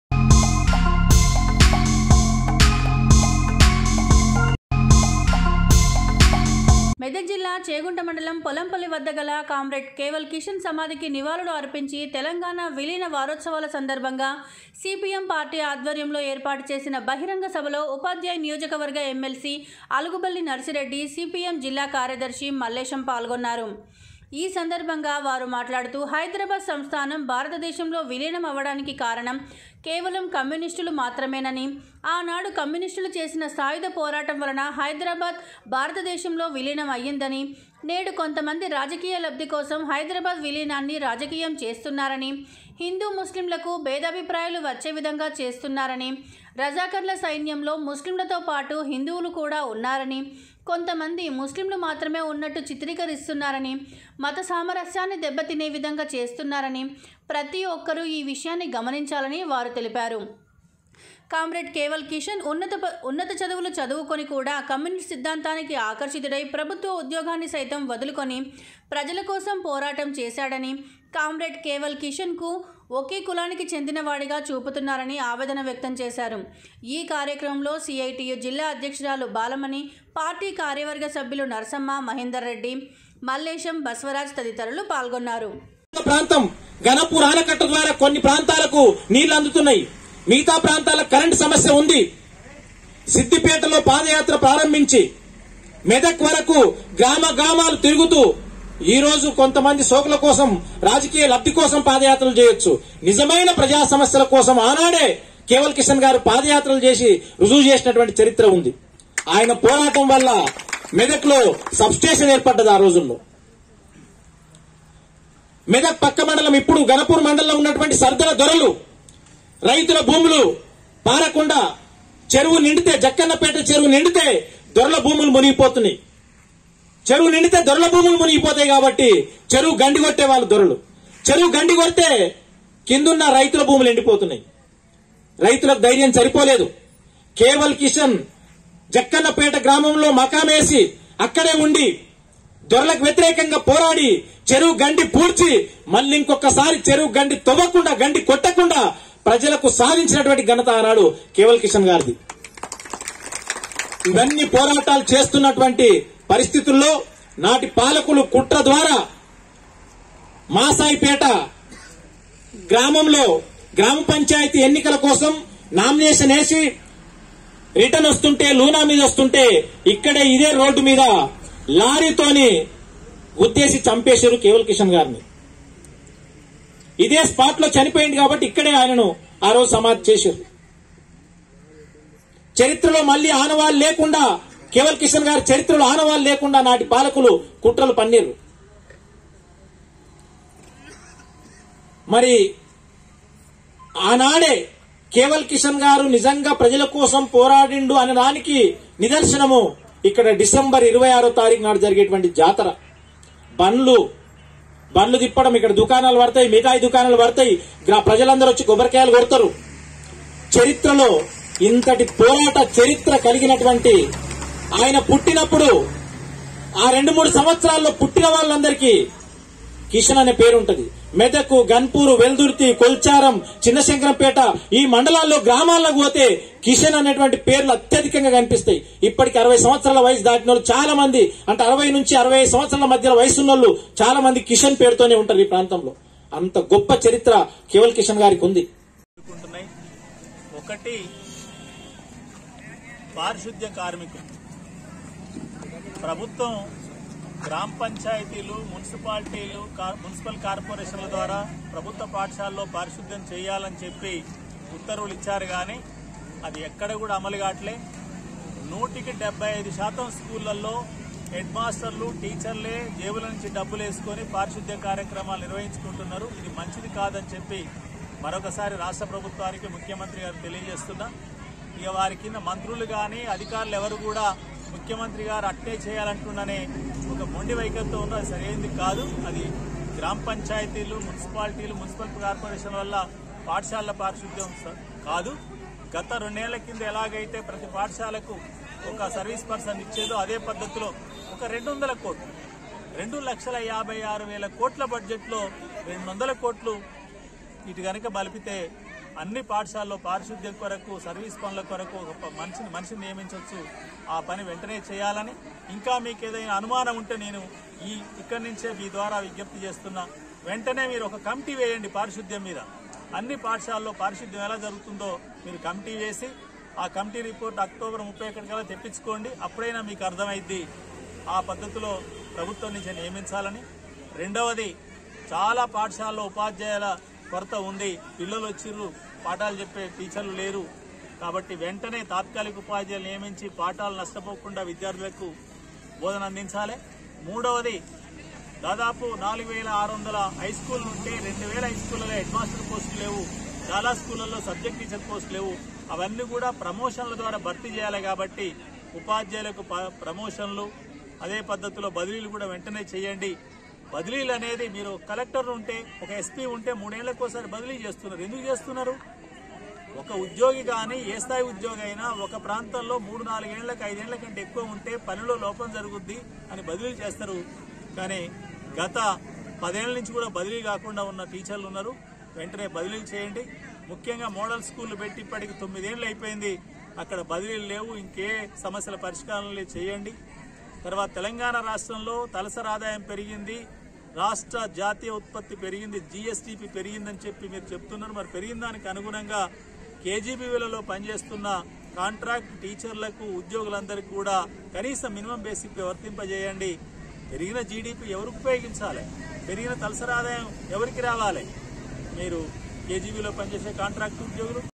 చేగుంట మండలం పోలంపల్లి వద్దగల కామ్రేడ్ కేవల్ కిషన్ సమదికి నివాళులు అర్పించరు
చేగుంట మండలం పోలంపల్లి వద్దగల కామ్రేడ్ కేవల్ కిషన్ సమదికి నివాళులు అర్పించి , తెలంగాణ విలీన వారోత్సవాల సందర్భంగా CPM పార్టీ ఆధ్వర్యంలో  ఏర్పాటు చేసిన బహిరంగ సభలో  ఉపాధ్యాయ నియోజకవర్గ ఎమ్మెల్సీ ఆలుగుబల్లి నర్సిరెడ్డి ,, సిపిఎం జిల్లా కార్యదర్శి మల్లేశం పాల్గొన్నారు